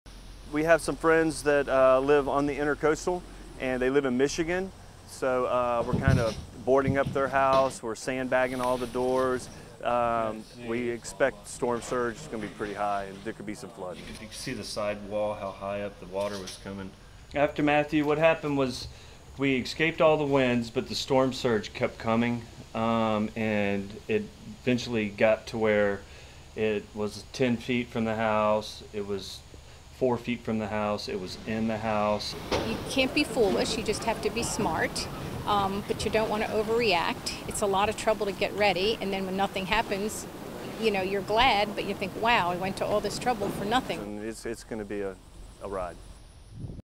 Florida's East Coast readies for Dorian
(2 Sep 2019) FOR CLEAN VERSION SEE STORY NUMBER: apus122874

From Florida to North Carolina, residents and government officials have been preparing for possible impacts of Dorian for days, even as the official forecast has the center of the storm staying offshore.
In Ponte Vedra Beach, Florida residents who have decided not to evacuate are sandbagging and boarding their homes ahead of Dorian's possible impact.
Jesse James is helping friends board up their home right next door to his former property that was lost to hurricane Matthew in 2016.
Just down the road residents are filling up sandbags, preparing for possible storm surge and hoping for the best in this low-lying area.
The Category 5 hurricane's nearly unprecedented strength — the 185 mph (295 kph) winds made it the second strongest storm in the Atlantic Ocean since 1950 — has people aware that even a tiny error in the forecast could be catastrophic.